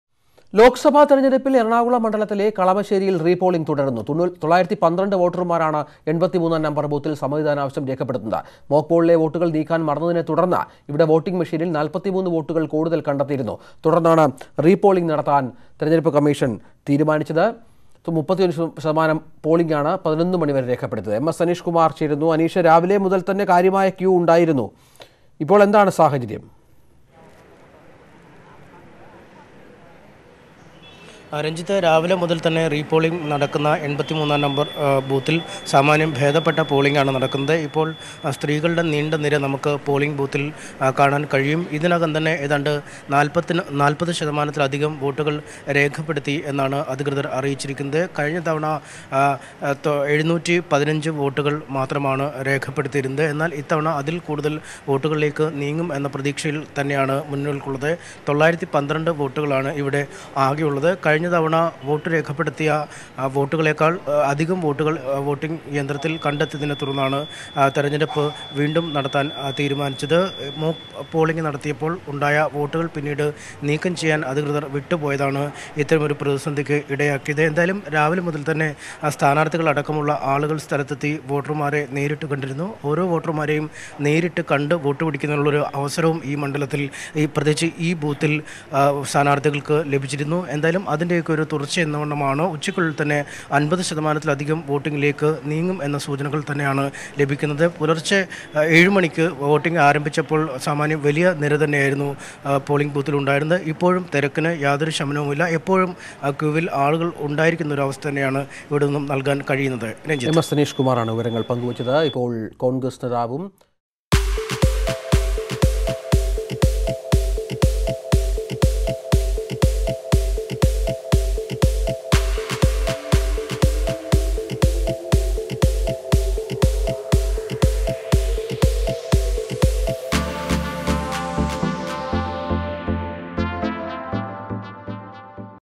കളമശ്ശേരിയിൽ റീപോളിംഗ് പുരോഗമിക്കുന്നു; മോക് വോട്ടുകൾ നീക്കം ചെയ്യാൻ വിട്ടുപോയതാണ് കാരണം
The Election Commission is holding a repolling in one booth in Kalamassery in Ernakulam Lok Sabha constituency.